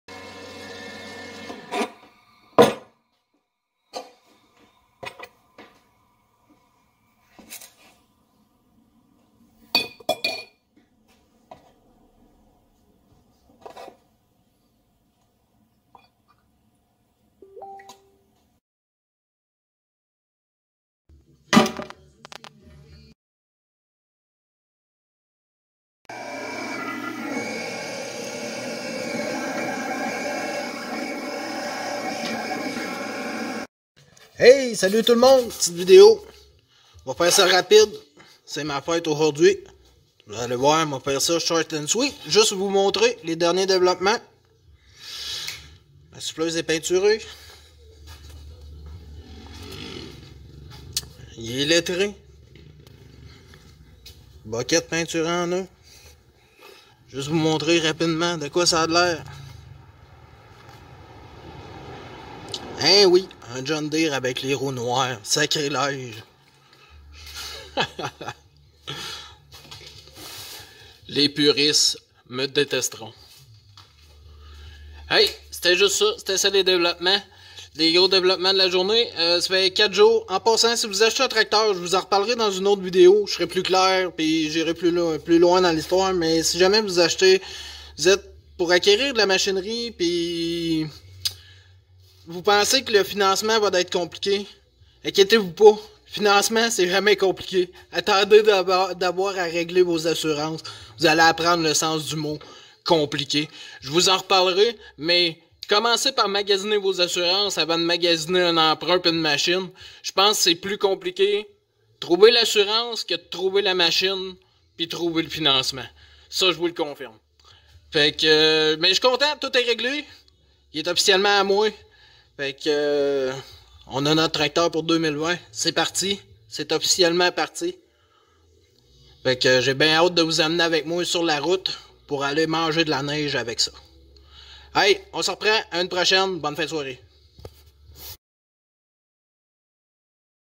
J'ai terminé l’esthétique !
Préparation de la saison 2020-21 . Déneigement au Québec